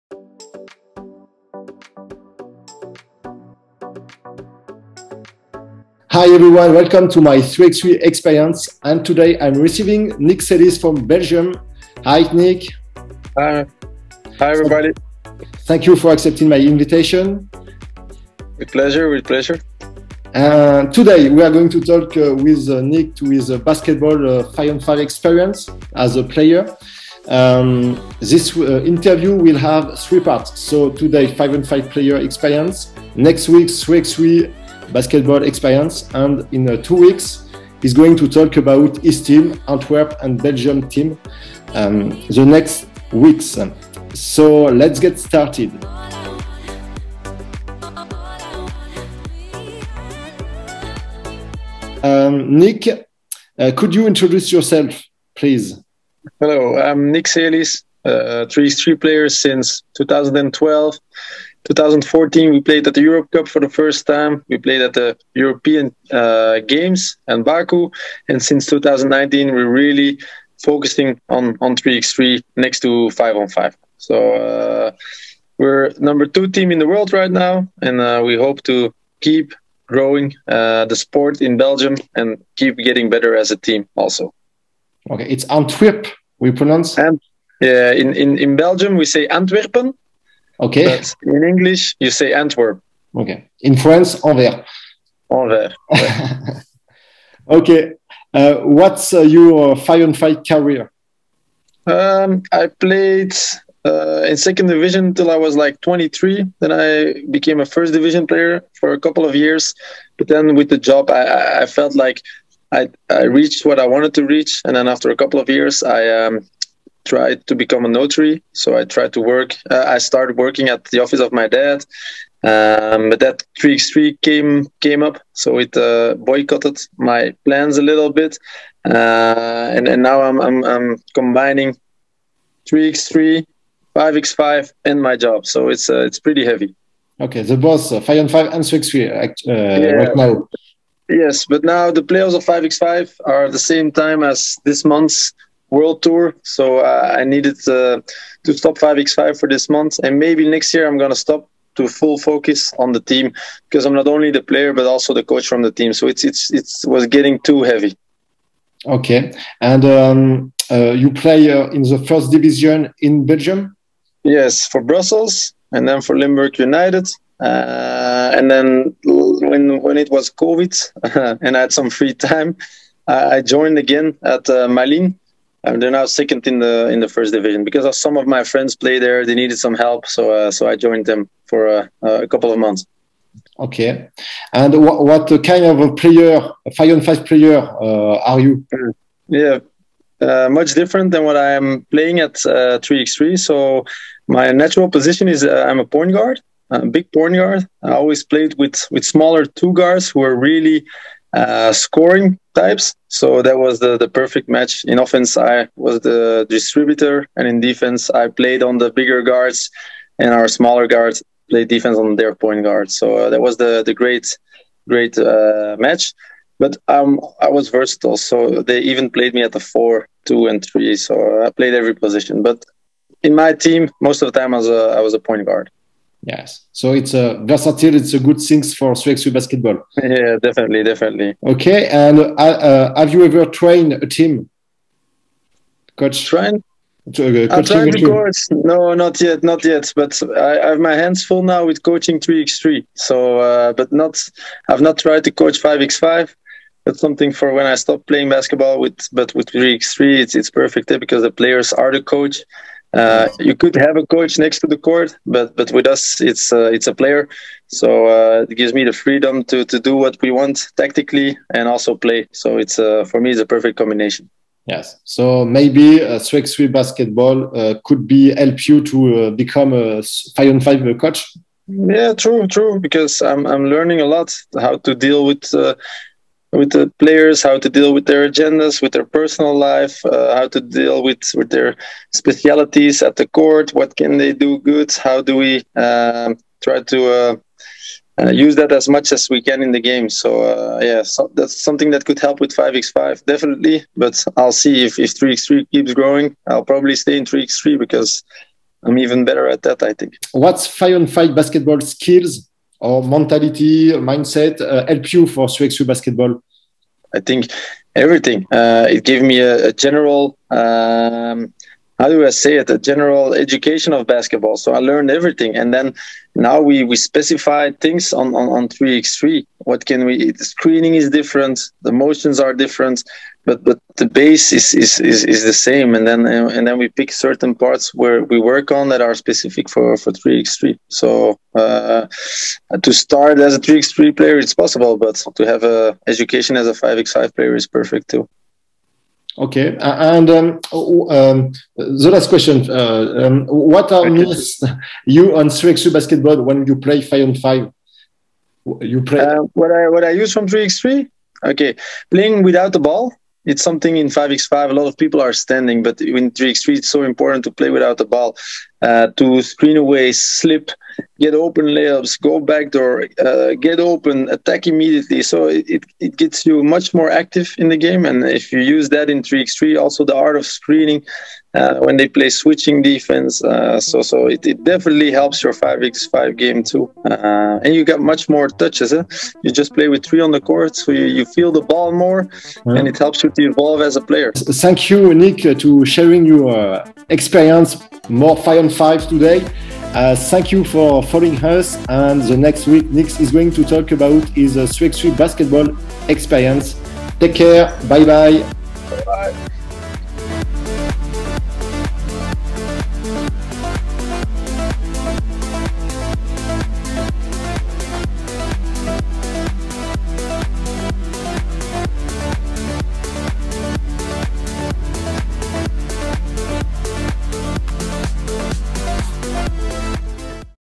3x3 basket-ball : Nick Celis 3x3 basket-ball player [Interview part 1]
In this video, we meet Nick Celis a 3x3 basket-ball player from Belgium.

In this first part : Who is Nick ?

My Book 👉BASKET 3X3: Fondamentaux, exercices et systèmes👈